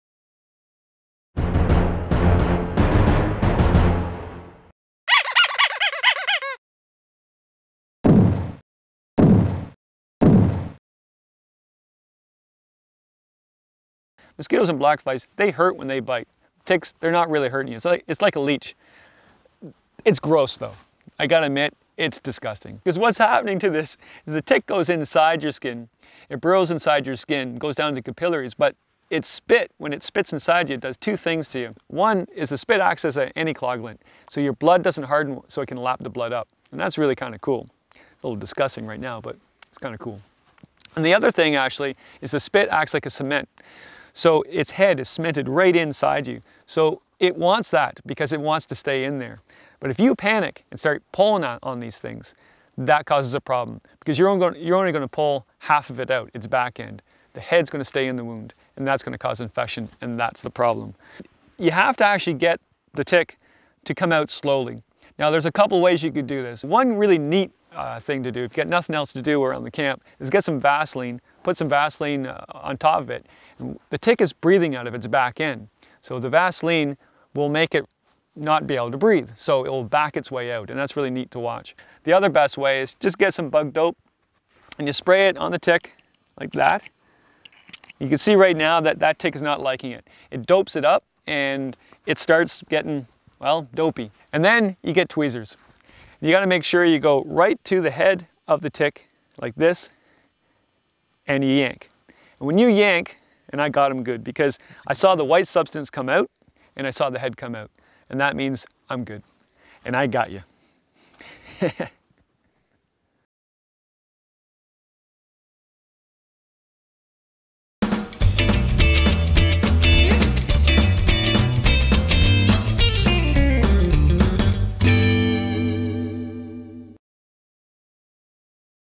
Dealing with Ticks
Happy Camper, Kevin Callan, shows how to safely remove a tick while out camping.